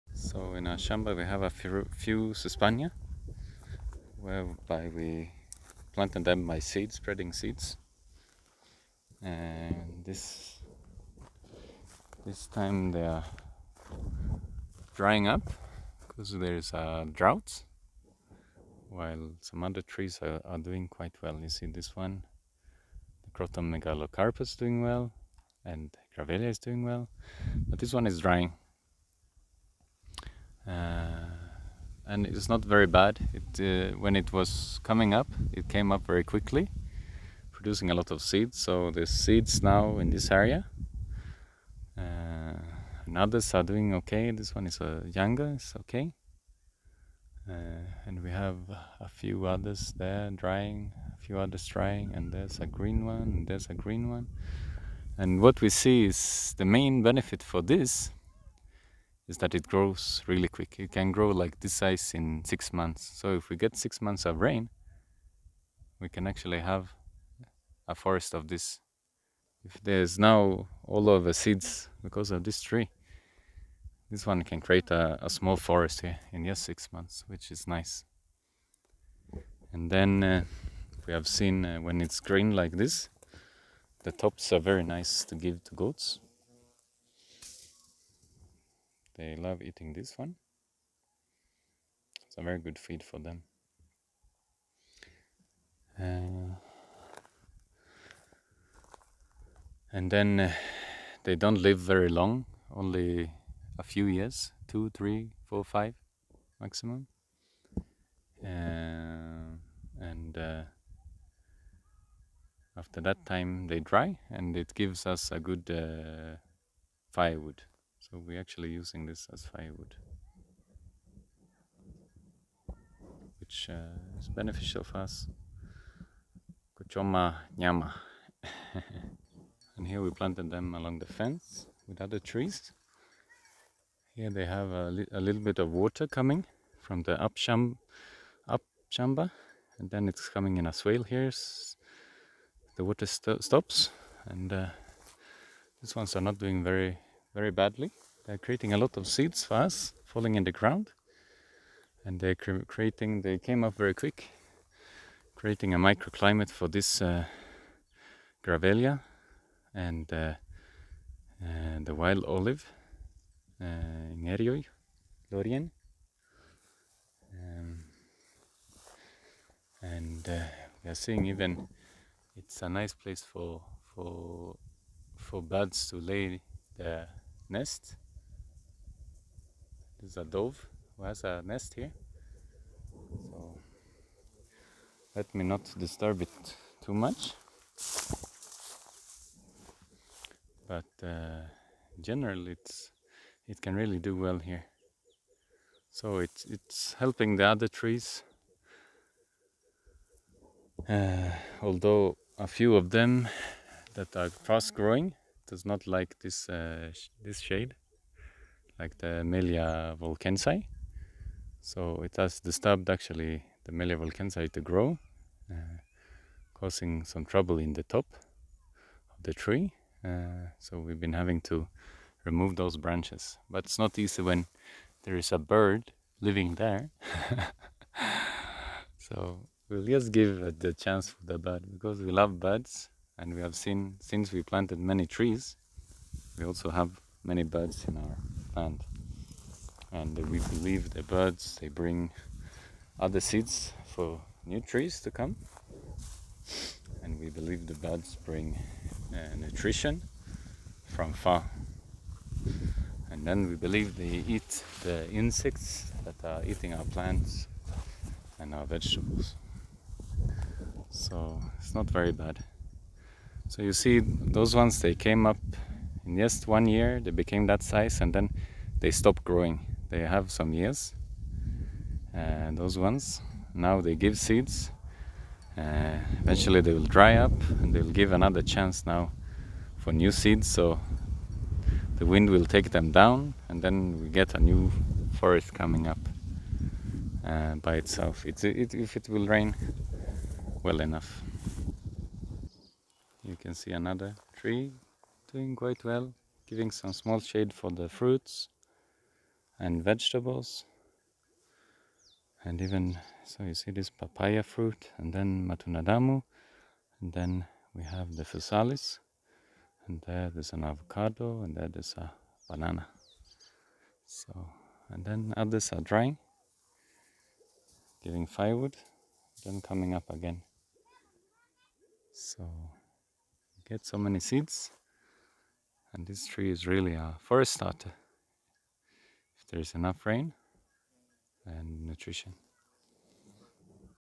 Sesbania Sesban: growing in semi arid land. Fast firewood production, chicken and goat feed.
Samburu name: Loyiangalani
Maasai name: oloyiangalani
Botanic name: Sesbania sesban

Agroforestry use:
General: Grows fast, not drought tolerant, nitrogen fixer, bird habitat and feed.

Flowers: nectar used by some polinators
Seeds: great ckicken feed
pods: great fire starter when dry
leaves: goat and cow feed
sticks and wood: firewood (when well dried it burs well with no smoke but charcoal content is small)